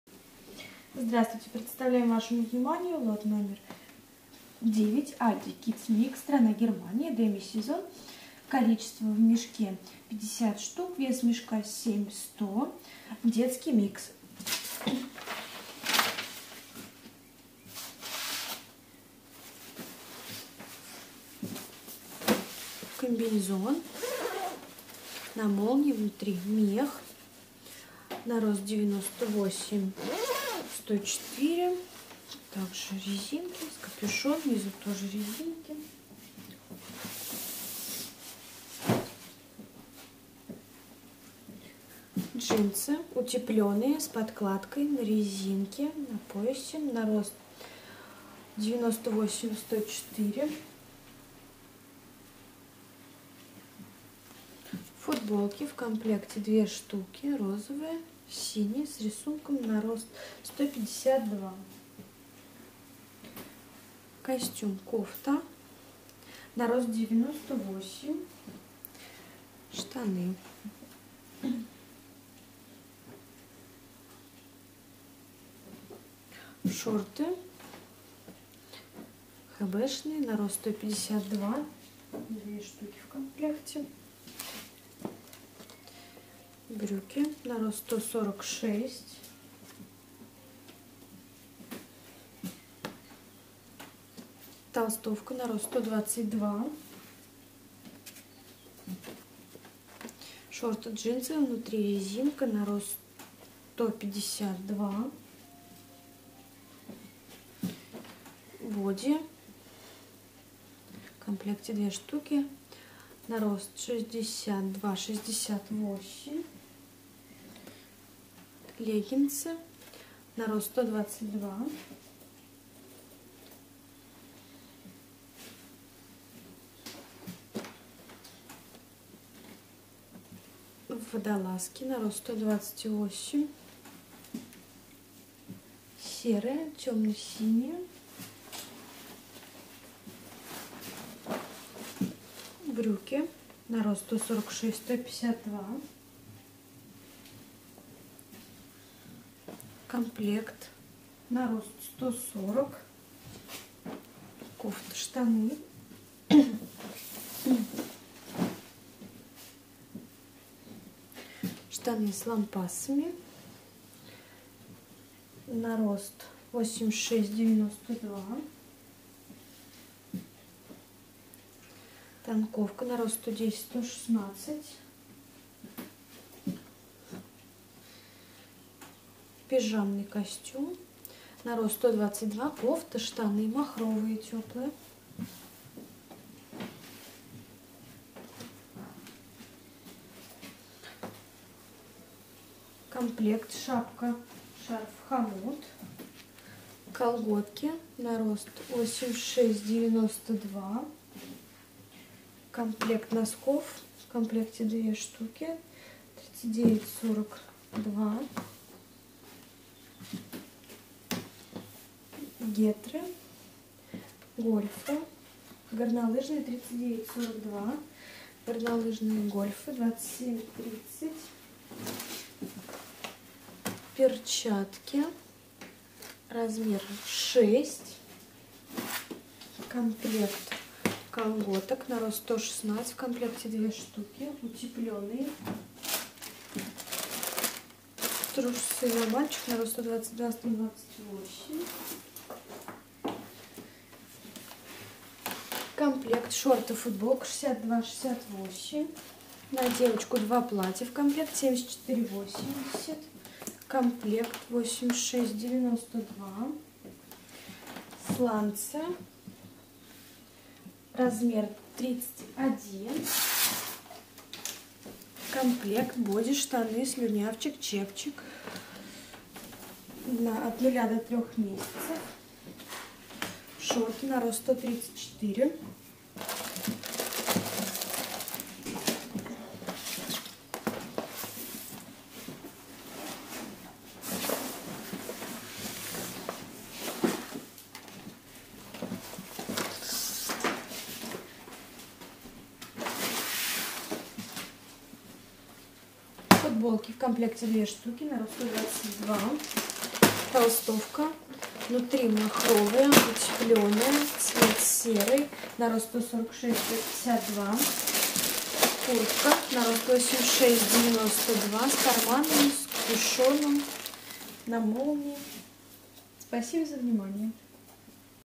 ALDI CRANE KIDS MIX 9,Цена за кг 9 евро ,cток одежда оптом
ALDI KIDS MIX  (лот №9)
Детский микс (сток )
Страна: Германия
Сезон: Демисезон
Цена за кг.: 9 евро
Цена за мешок: 63.9 евро
Цена за ед.: 1.27 евро